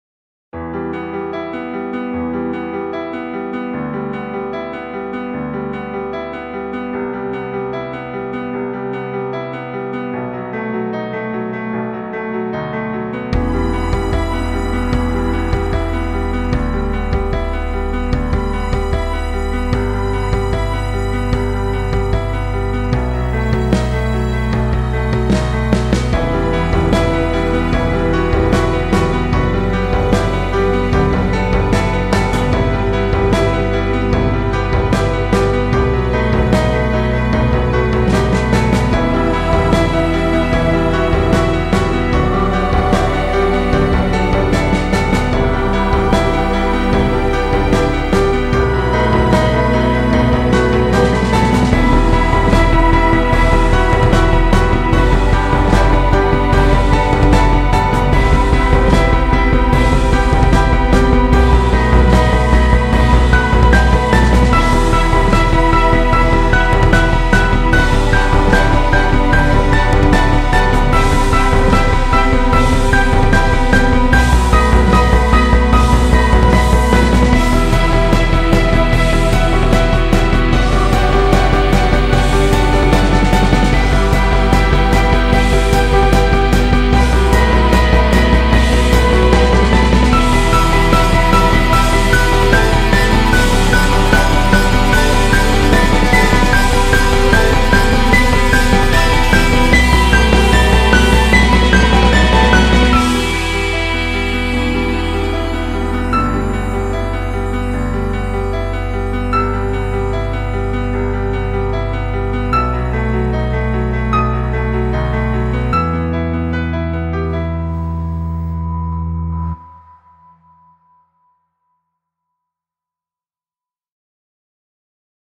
Underworld Music
Dark cinematic suspence music gradually build to loud final, dramatic and sad. The ultimate emotive strings and beats combination. Music full of strings and choir, teaser and trailer available. . Instrumental. Synchronization. musicforvideo backgroundmusic muzaproduction synchronization# royaltyfreemusic